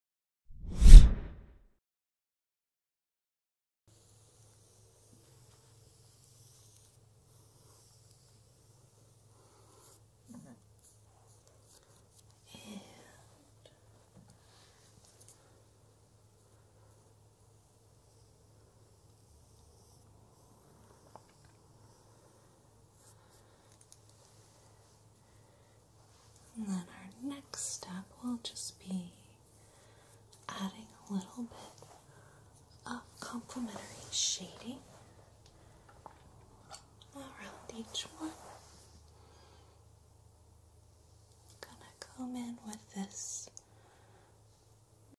Pat ASMR Double Back Painting Preview
Corrina talks in soothing whispers and paints on two lovely models.